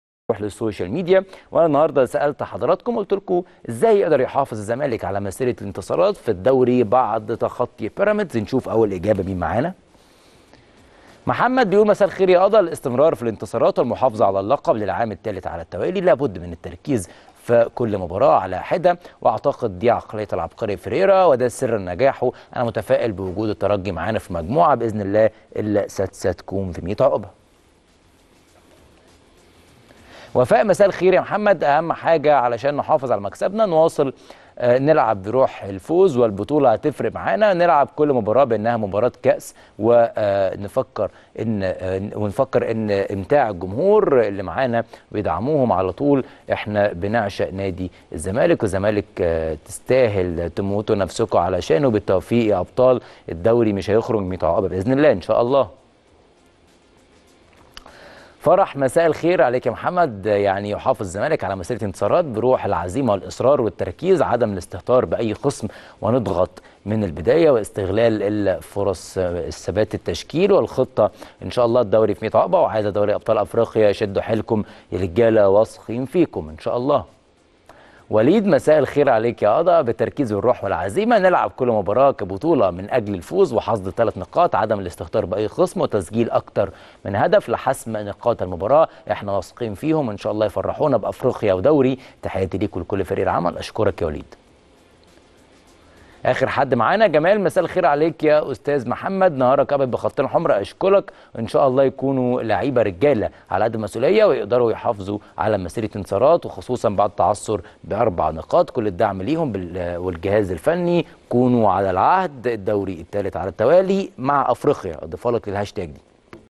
آراء الجمهور في موضوع الحلقة.. كيف يحافظ الزمالك على مسيرة الإنتصارات في الدوري بعد تخطي بيراميدز
آراء الجمهور في موضوع الحلقة.. كيف يحافظ الزمالك على مسيرة الإنتصارات في الدوري بعد تخطي بيراميدز - أخبارنا